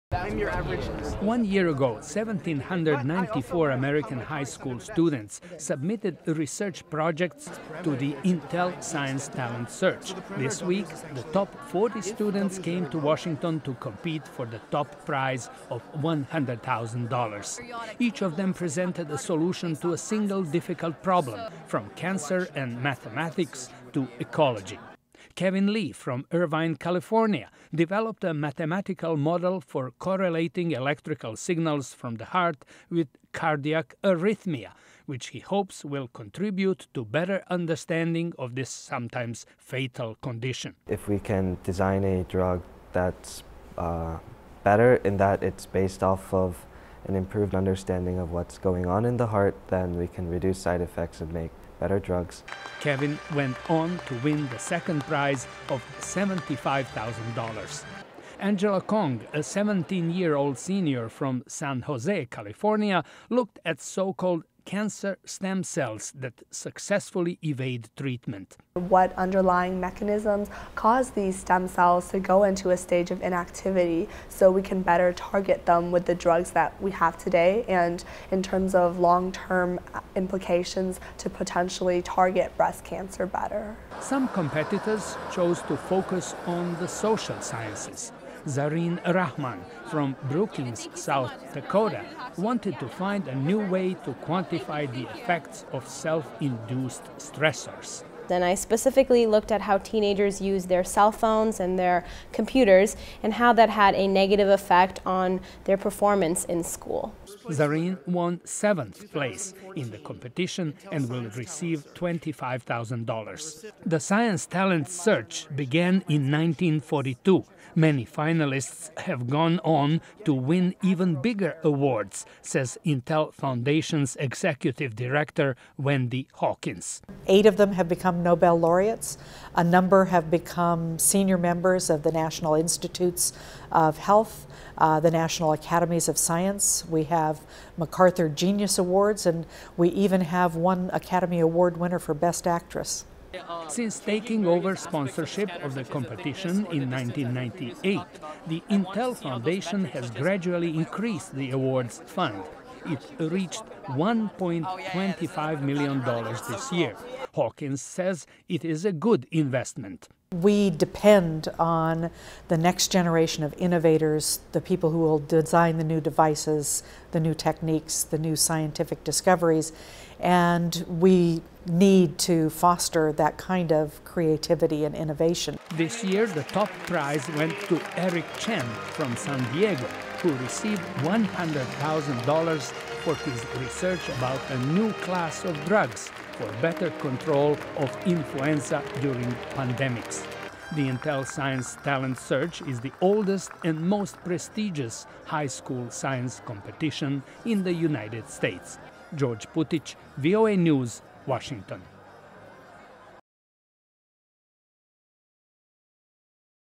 Intel Awards Top Prizes to Young Scientists
The United States may not be at the top of the world's rankings when it comes to test results for high school students, but that does not mean it lacks talent. VOA's George Putic talked with three of the young scientists.